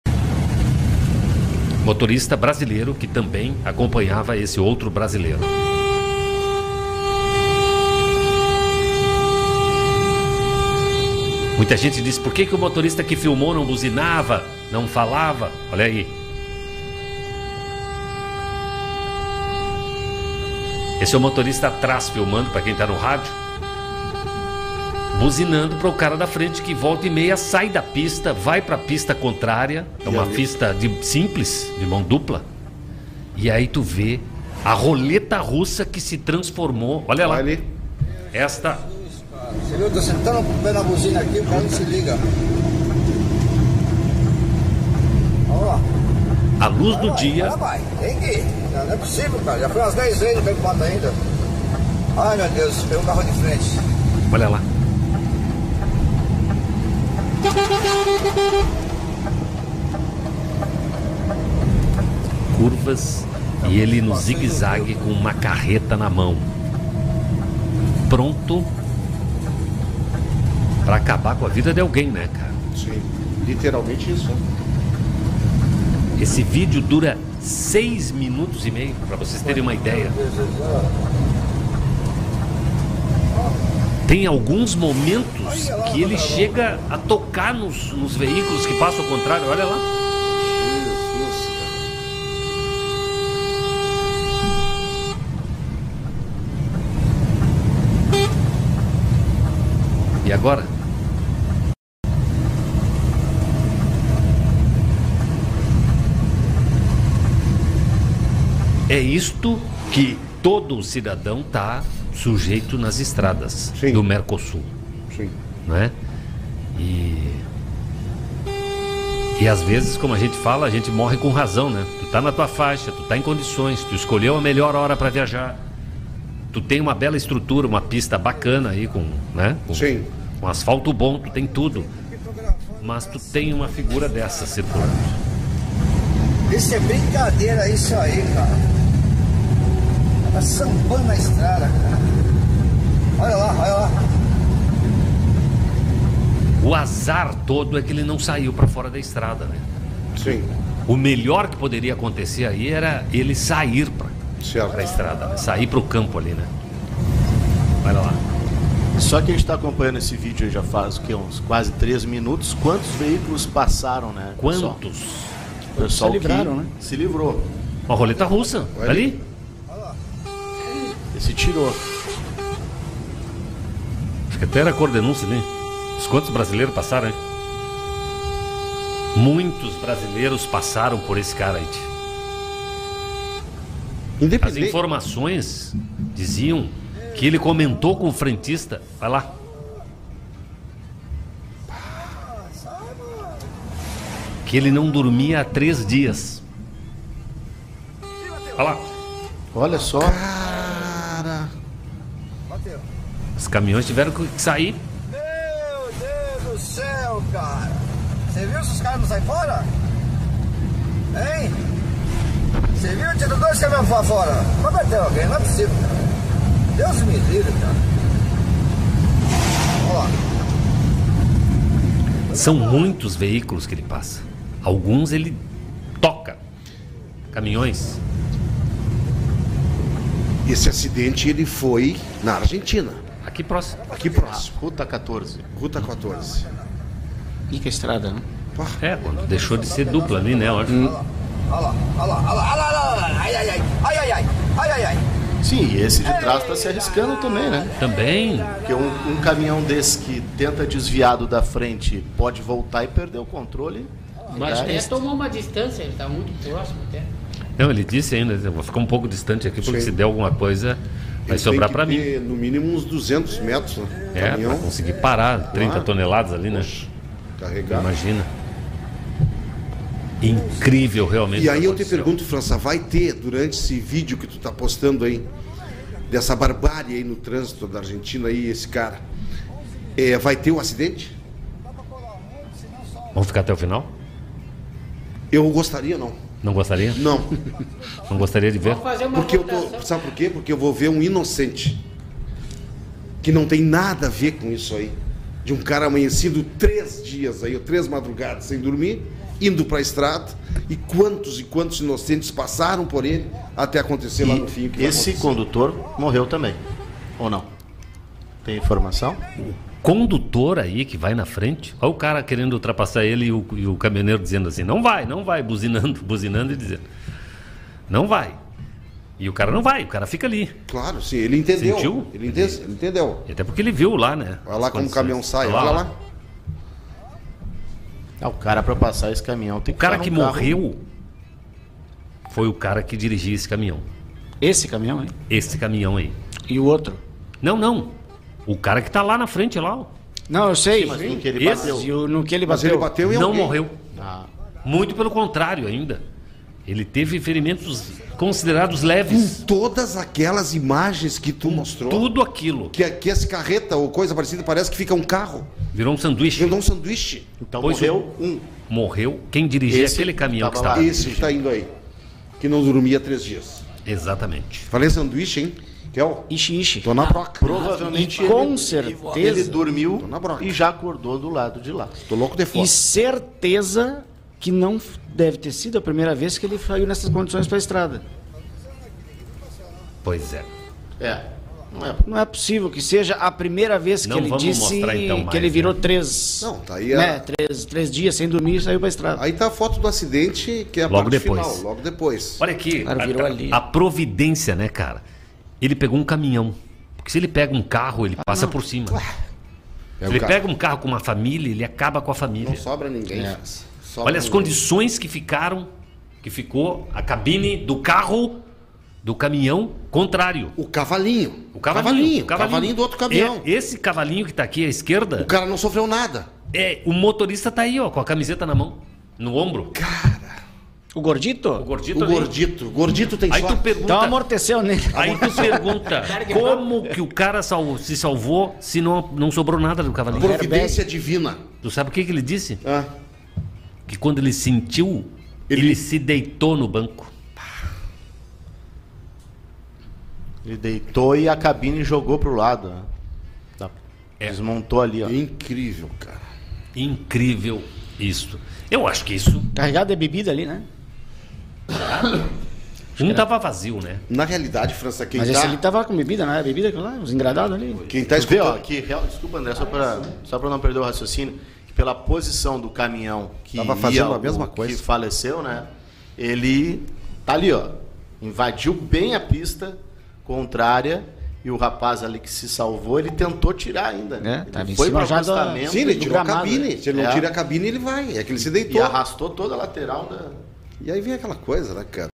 Motorista de caminhão viaja quase 300 km sem condições, e causa acidente fatal
Vídeo feito por motorista na Ruta 14 na Argentina, próximo ao fronteira como Brasil